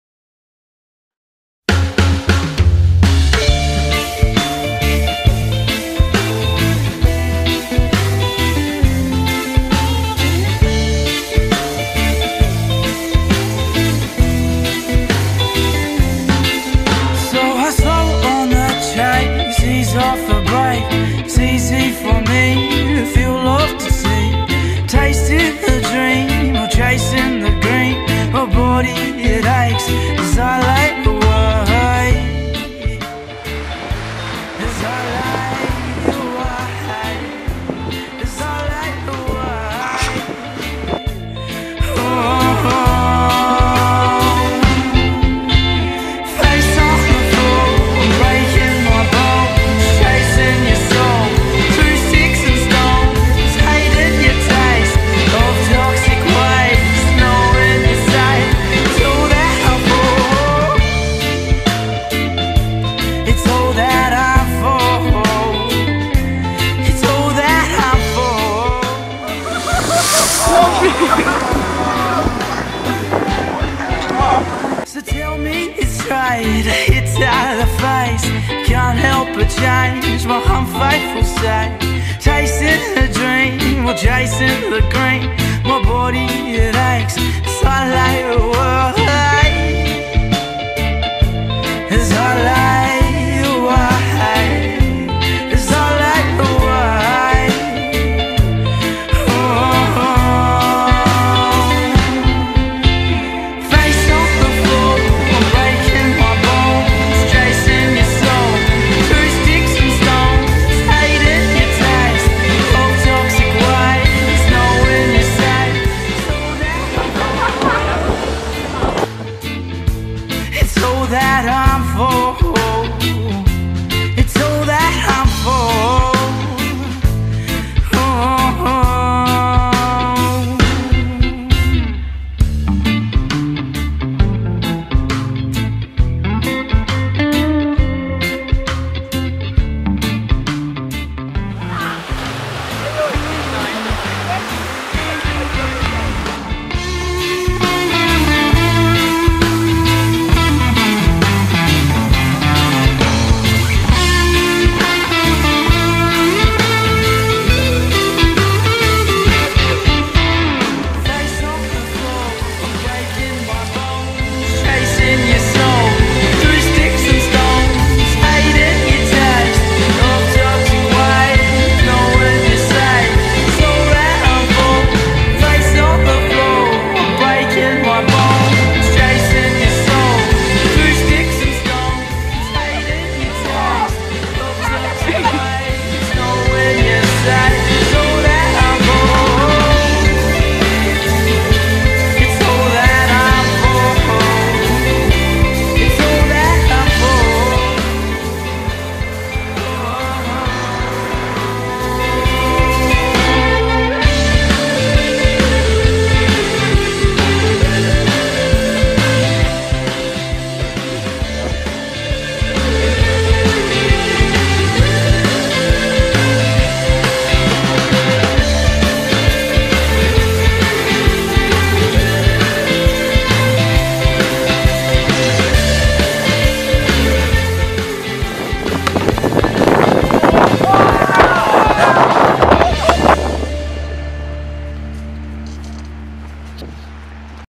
First adventure of 2018 // Port Elliot trip
2018 // Port Elliot trip.

Hey guys, here is my first take at filming and editing a video of the yearly holiday trip to Port Elliot, SA. Hope you all enjoy it and let me know what you think of it! More to come in the future hopefully! Want to give a shout out to anyone who was included in the making of the film, appreciate it. Leeeeeeeee whatttt?